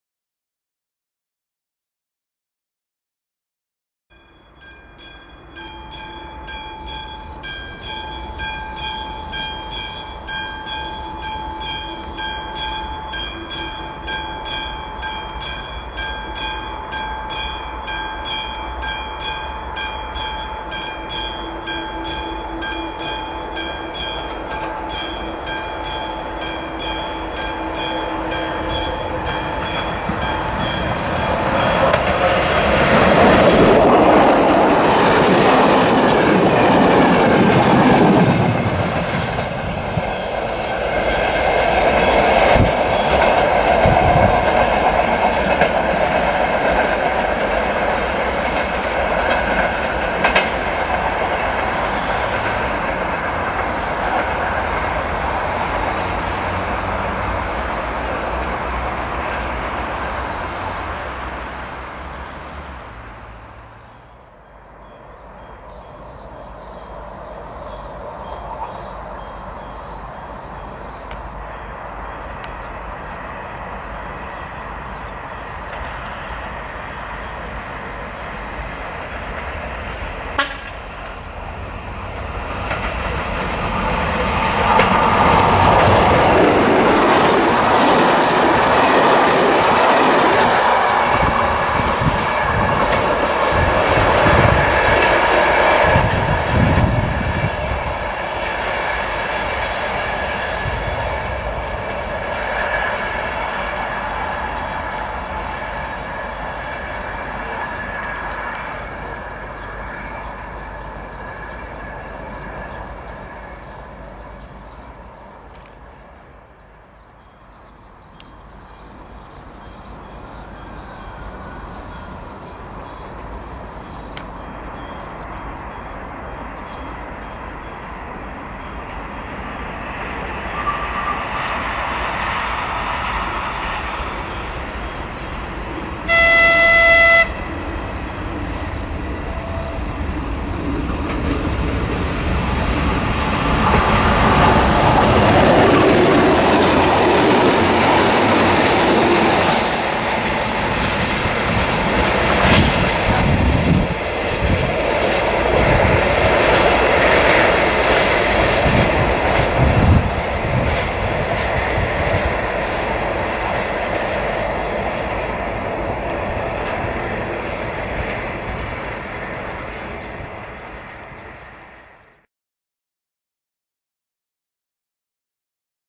České Budějovice-Nemanice ,řada 363 / 242 / 230 [ Full HD ]
Ráno v Nemanicích.
řada 363 s rychlíkem z Prahy
řada 242 s rychlíkem z Brna
řada 230 s nákladem